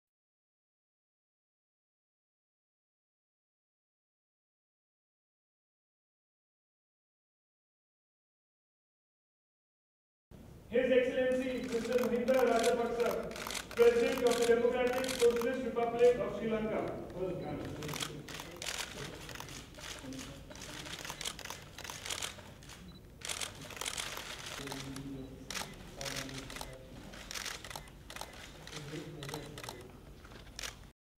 President Mahinda Rajapaksa of Sri Lanka called on President Mukherjee at Rashtrapati Bhavan
President Mahinda Rajapaksa of the Democratic Socialist Republic of Sri Lanka called on President Mukherjee at Rashtrapati Bhavan